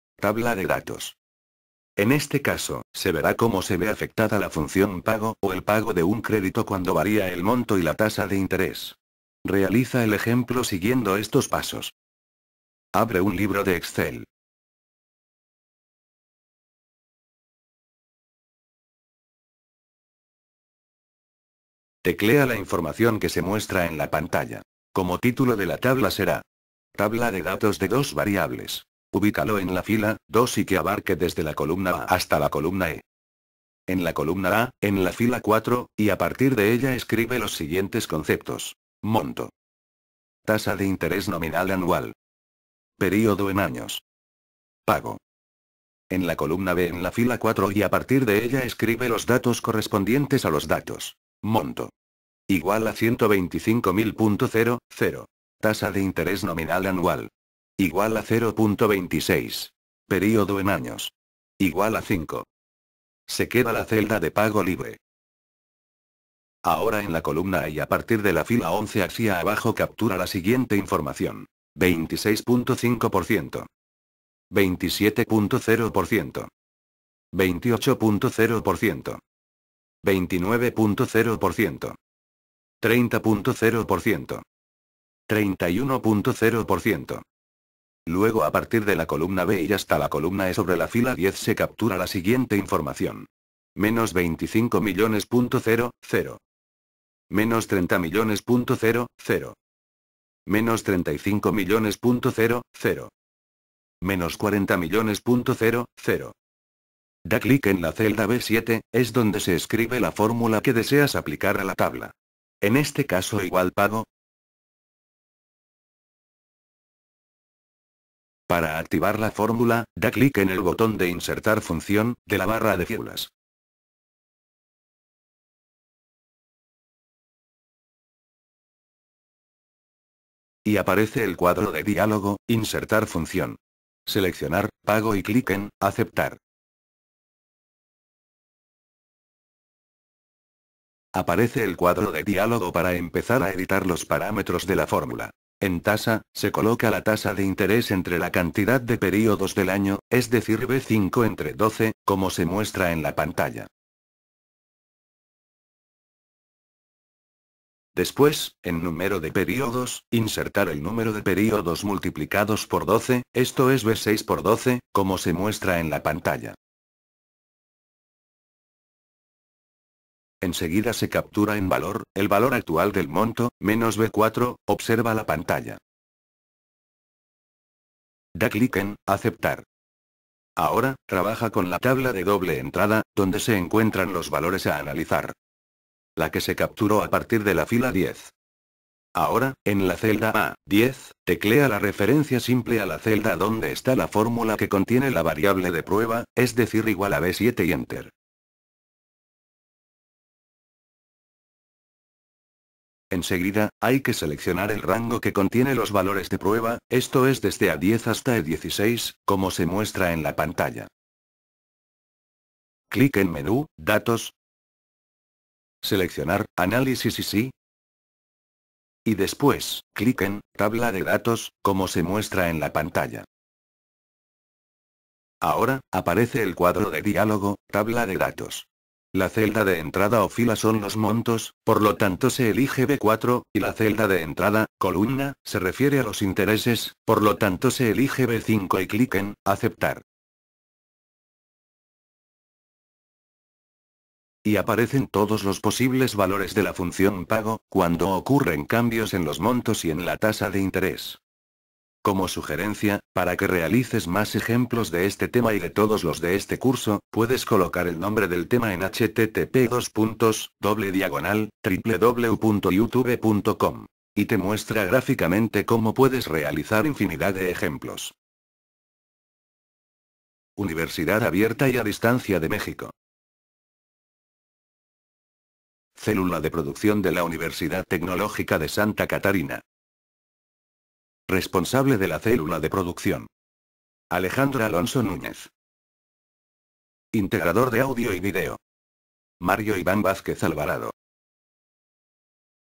Tabla de datos
Universidad Abierta y a Distancia de México
Distrito Federal, México, 2013
Cápsula educativa/Curricular/ Desarrollo Comunitario/4° Cuatrimestre/ Informática para la administración/ Unidad: 3 Excel avanzado/ Tema: 3.3. Herramientas de datos/Subtema: 3.3.1. Tabla de datos